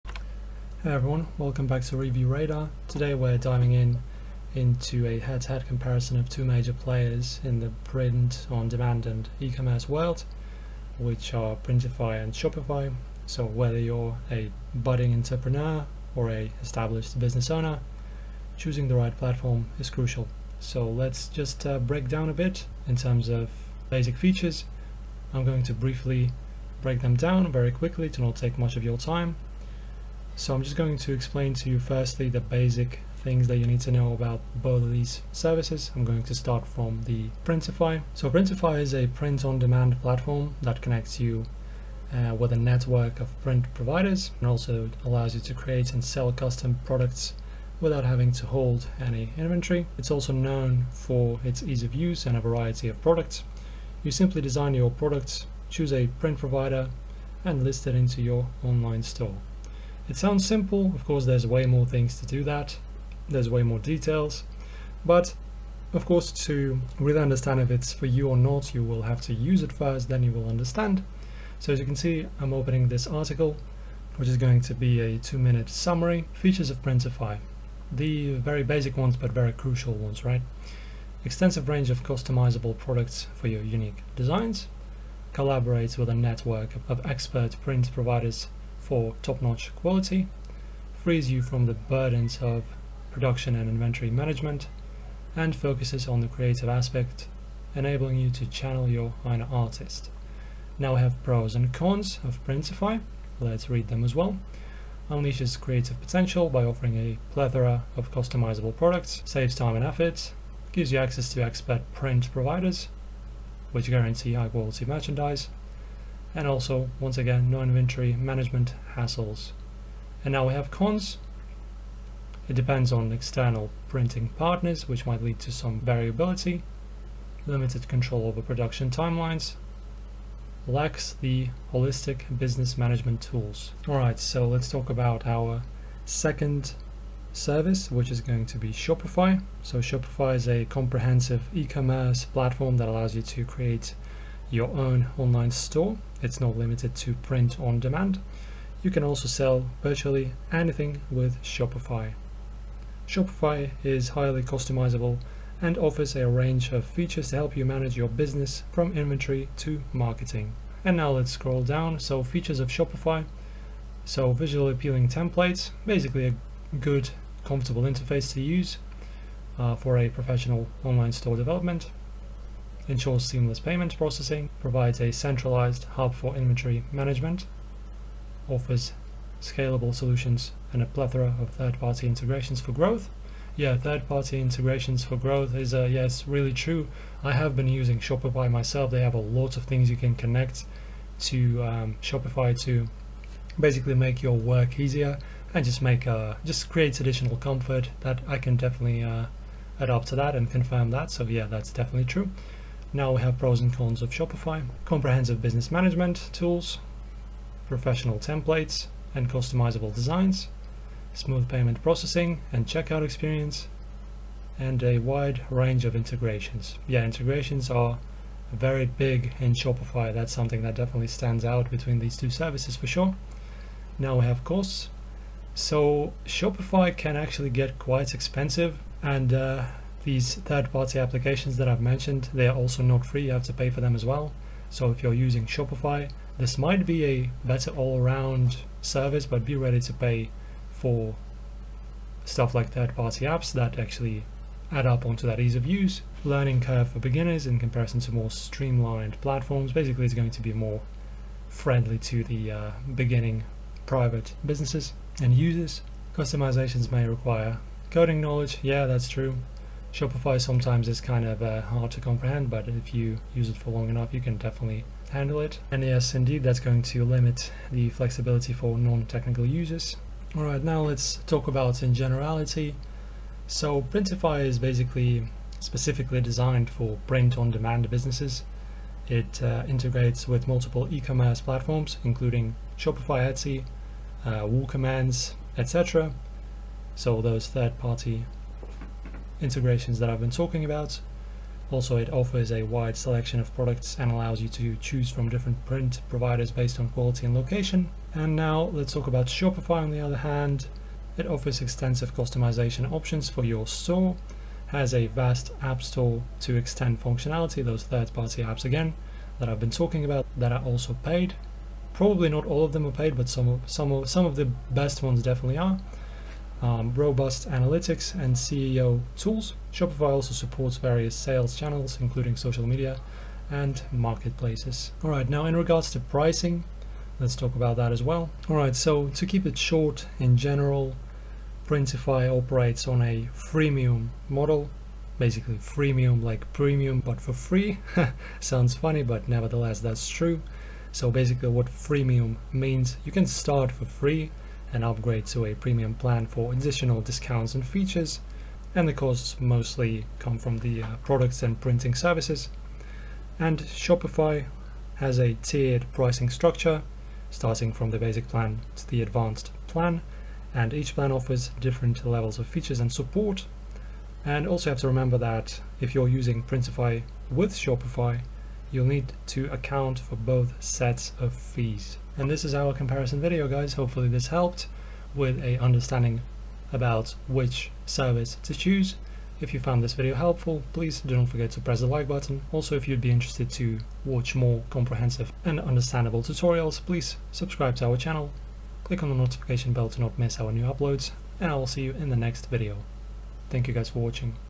Shopify vs Printify: Which is better? (2025)
In this video I compare the two webshop hosting services shopify vs printify.